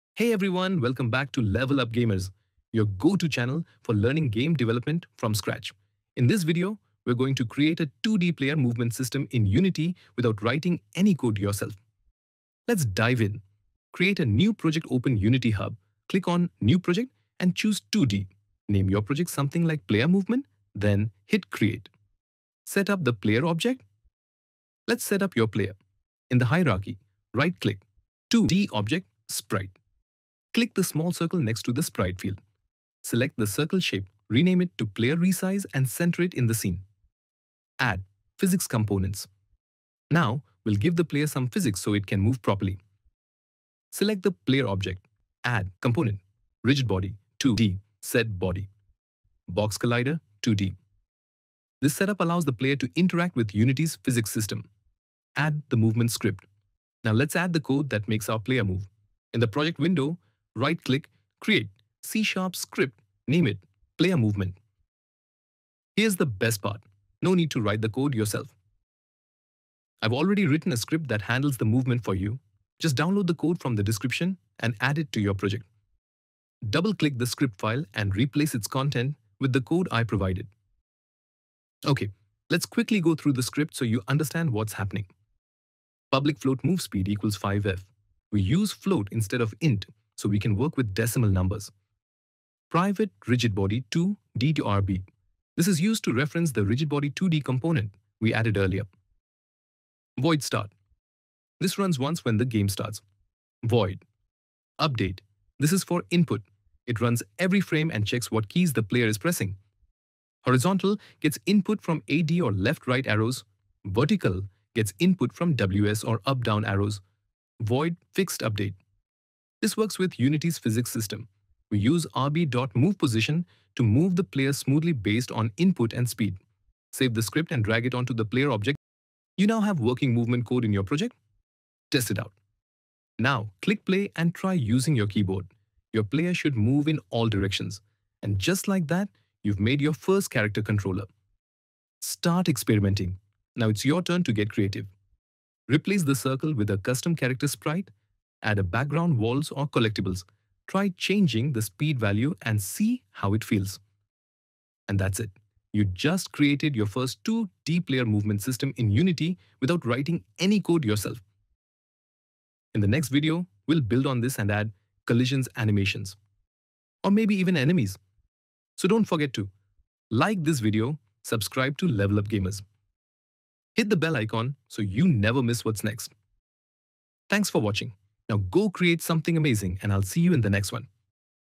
Unity 2D Player Movement (NO CODING) – Beginner Friendly Tutorial | Game Dev for Beginners
🚀 Learn how to create a 2D Player Movement system in Unity — without writing a single line of code!

This beginner-friendly tutorial is perfect if you're just starting your game development journey. We've prepared the movement script for you, so you can plug it into your Unity project and focus on learning how everything works — step by step.

✅ What You’ll Learn:
- How to set up a new 2D Unity project
- Adding Rigidbody2D and Collider2D components
- Plug-and-play movement script (no coding required!)
- Smooth character movement using keyboard input
- How Unity’s physics system interacts with your player

🧠 No prior coding or Unity experience needed!

🎮 By the end of this video, you’ll have a basic 2D character controller ready — and you’re one step closer to making your first game!

🔥 Next Video Teasers:
- Adding collisions and animations
- Creating enemies and interaction
- Full 2D platformer series coming soon!